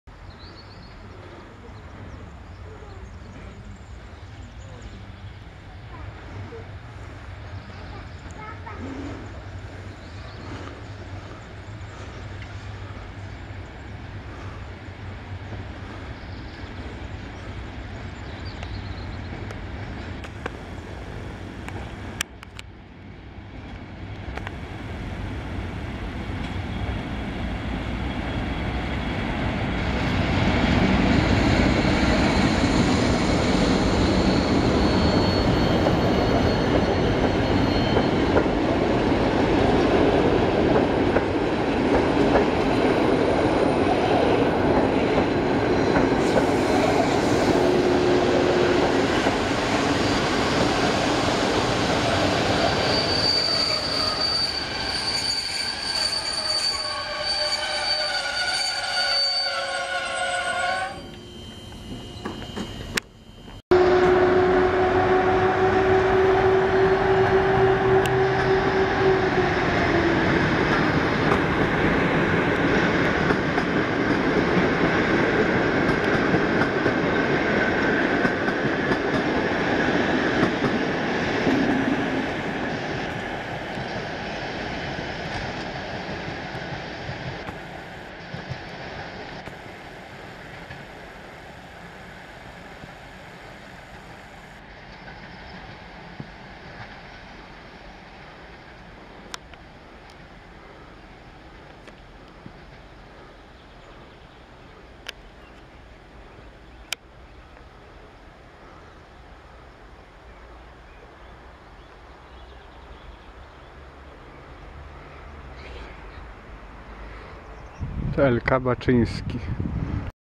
EP07-1026 z TLK Baczyński.
Przyjazd i odjazd ze stacji Łowicz Przedmieście w str. Łodzi.